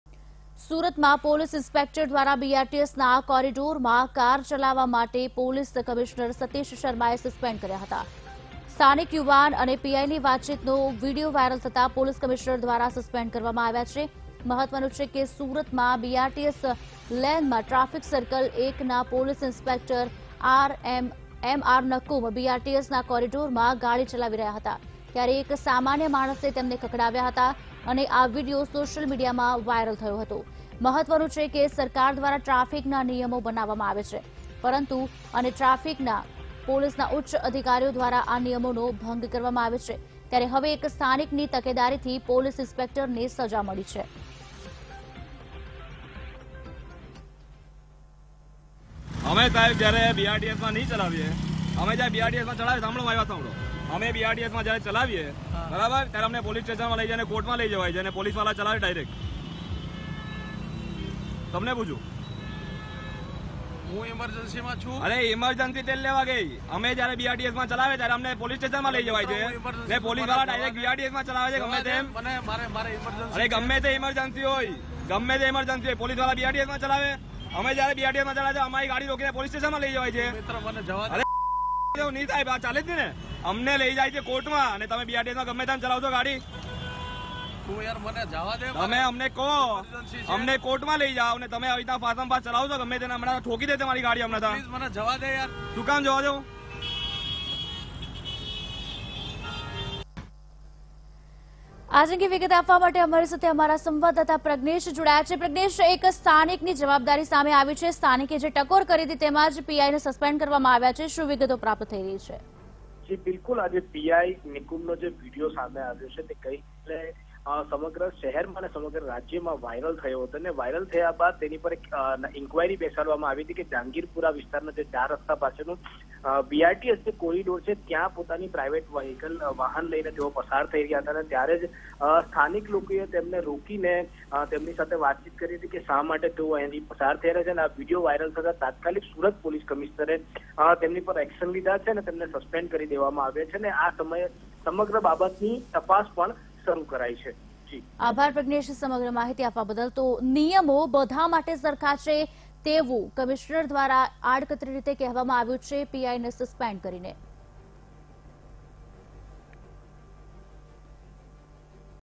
Surat: PI suspended after bring scolded by common man for driving in BRTS lane | Vtv News
Surat: PI suspended after bring scolded by common man  for driving in BRTS lane